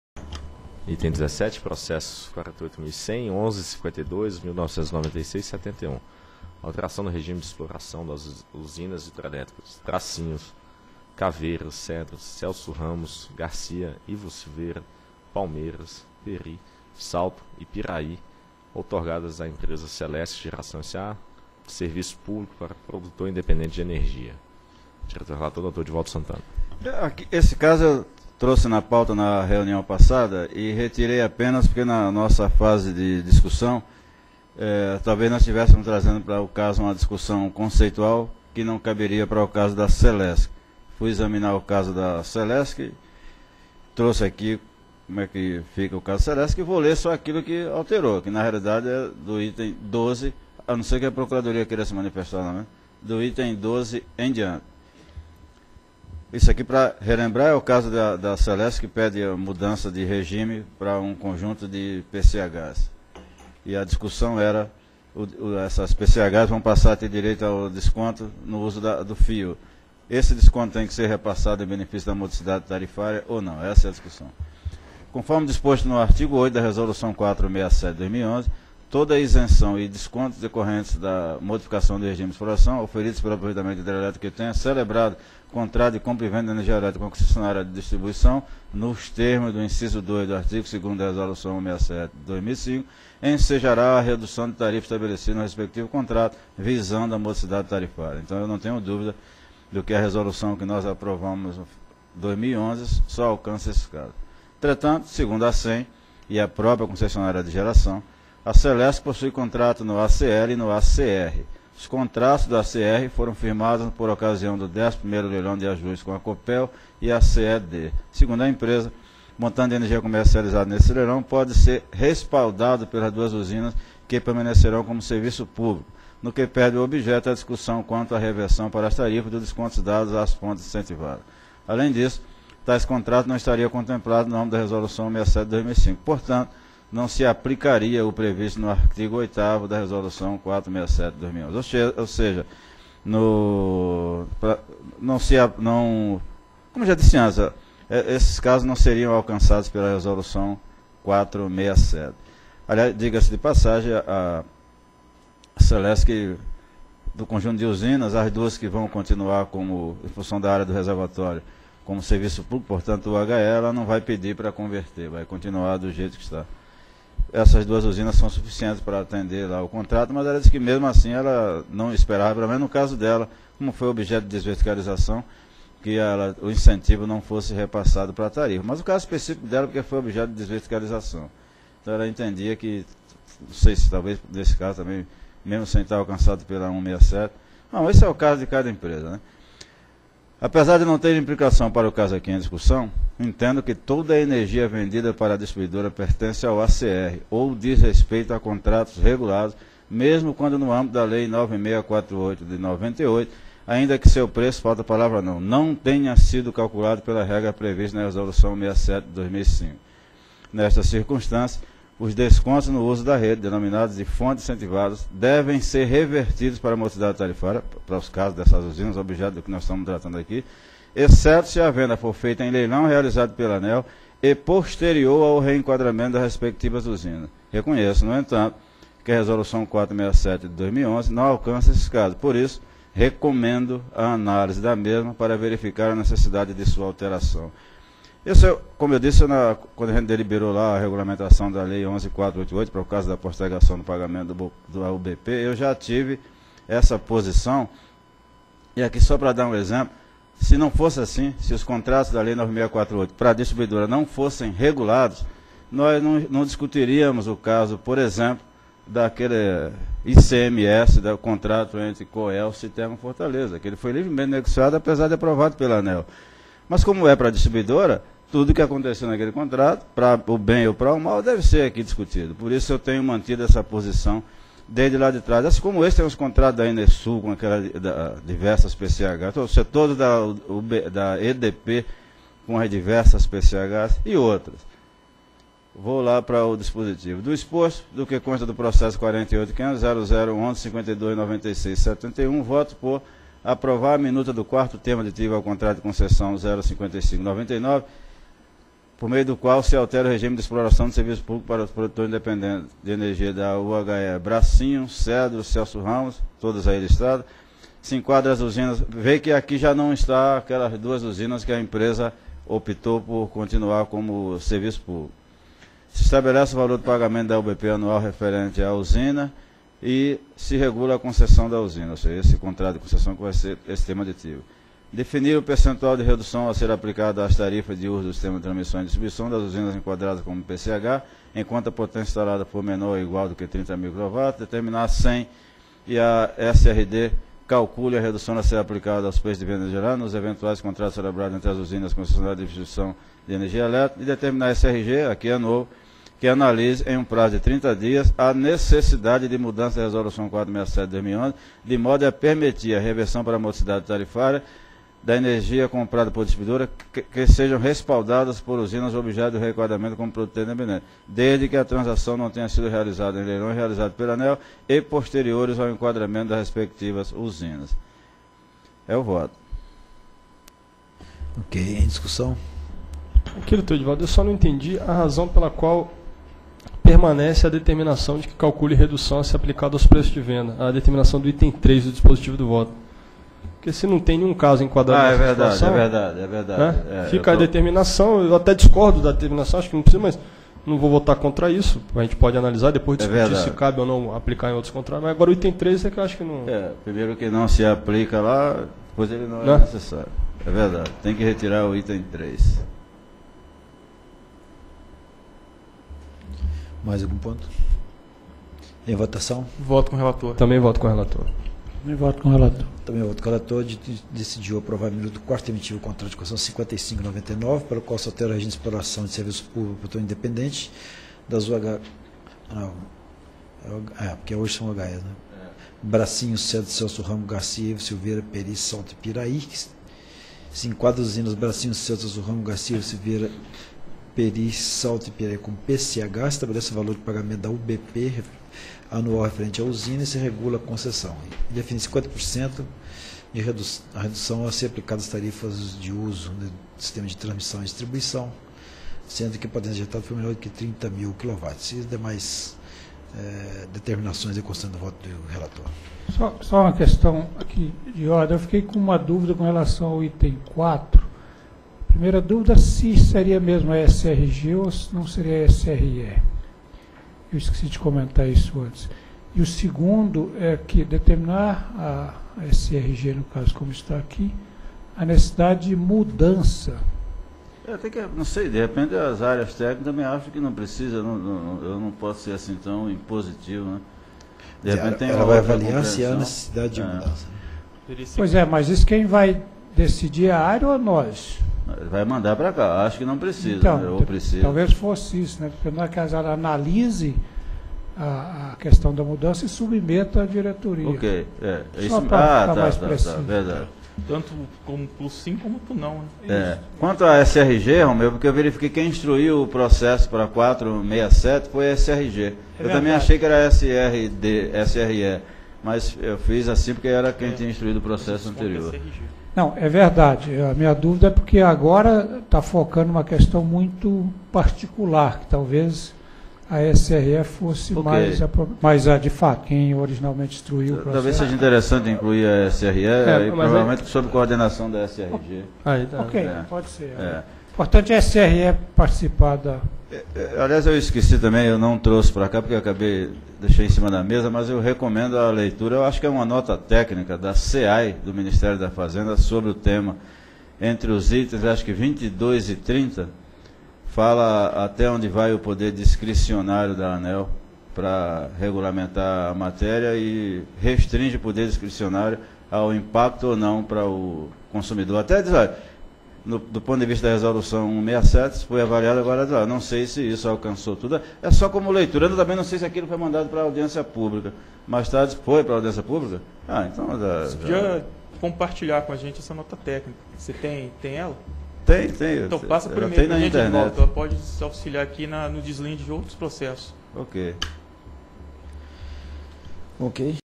Item 17 - 15ª Reunião Pública Ordinária ANEEL 02/05/2012
Item 17 - Processo nº: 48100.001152/1996-71. Assunto: Alteração do regime de exploração das Usinas Hidrelétricas -- UHEs Bracinho, Caveiras, Cedros, Celso Ramos, Garcia, Ivo Silveira, Palmeiras, Pery, Salto e Piraí, outorgadas à Celesc Geração S.A., de serviço público para Produtor Independente de Energia Elétrica -- PIE. Relator: Diretor Edvaldo Alves de Santana.